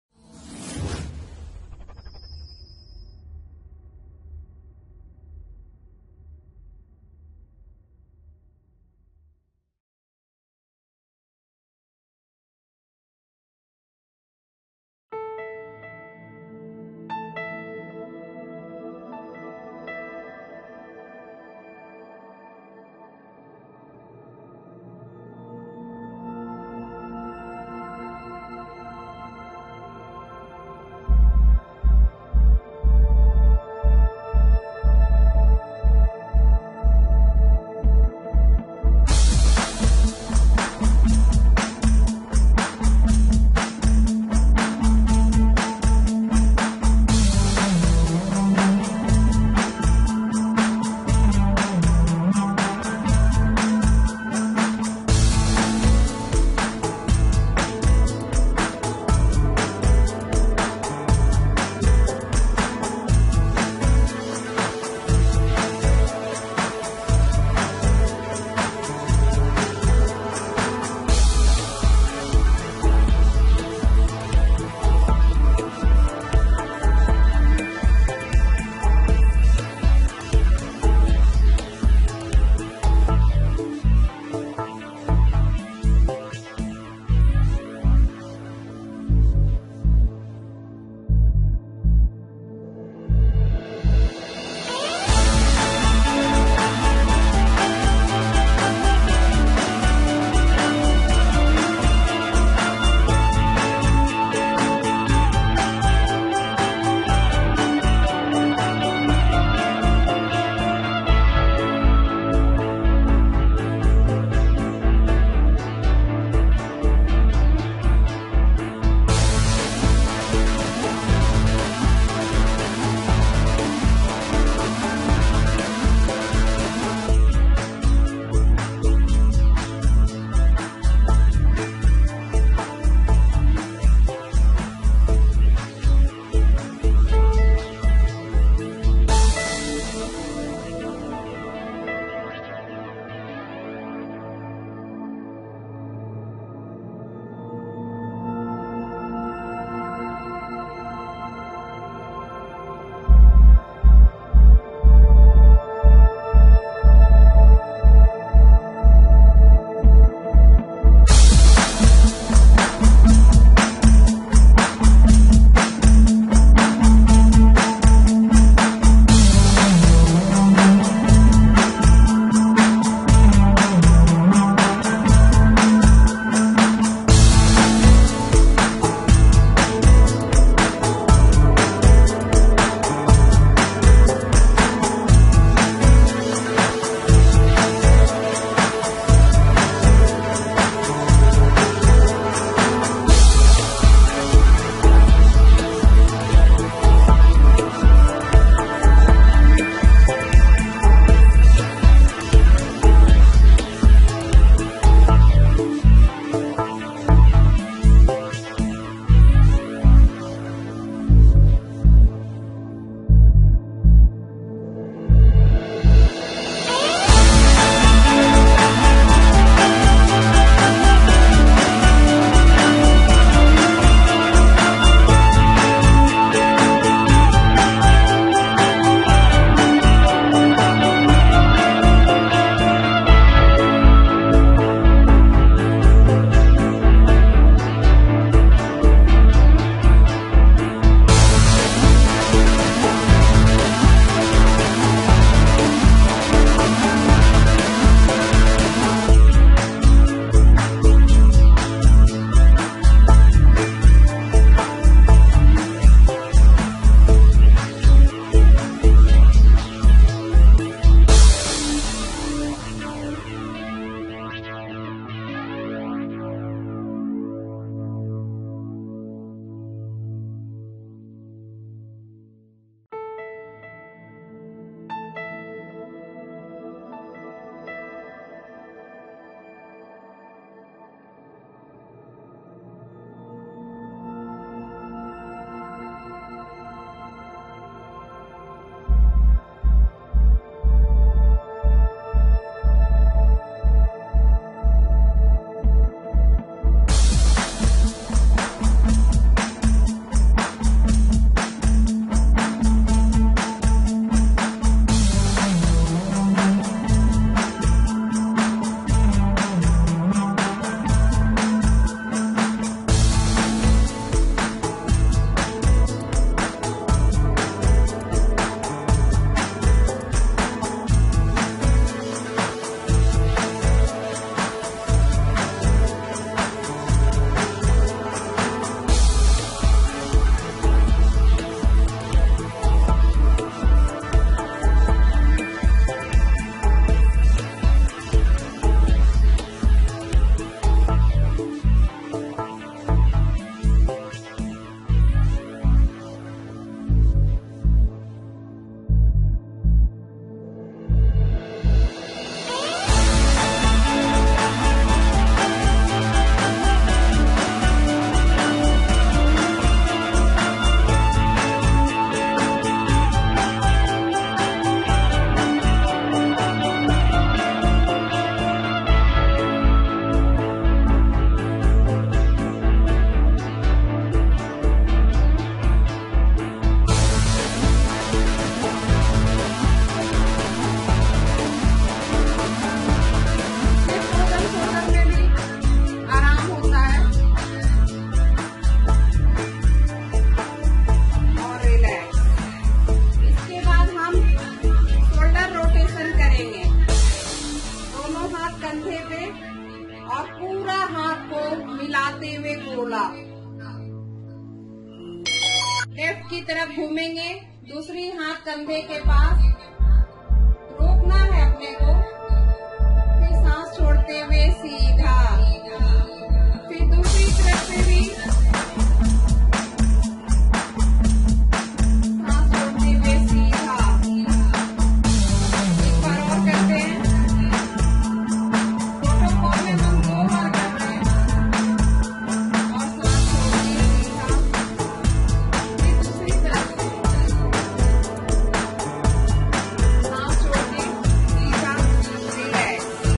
नोएडा विश्व योग दिवस की झलक। आनलाईन और आफलाईन। पतंजलि महिला योग समिति।
@vatankiawaz नोएडा योग दिवस पर योग साधकों ने आनलाईन और आफलाईन योग किया। बड़े संख्या मे लोगों ने अपने अपने घरों मे रहकर स्वामी रामदेव जी के साथ और स्वयं एक आनलाईन समूह बनाकर योग अभ्यास किया।